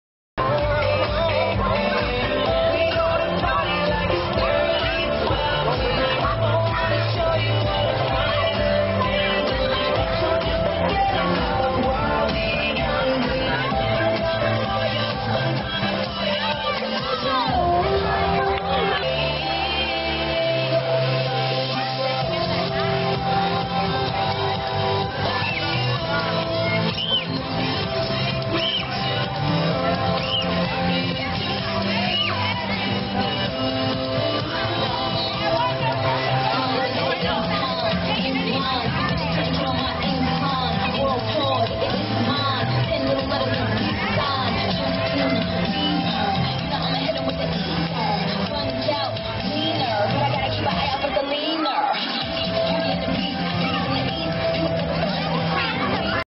EL BAILE DE GISELLE 2da PARTE
Giselle en otro momento del baile de su Escuela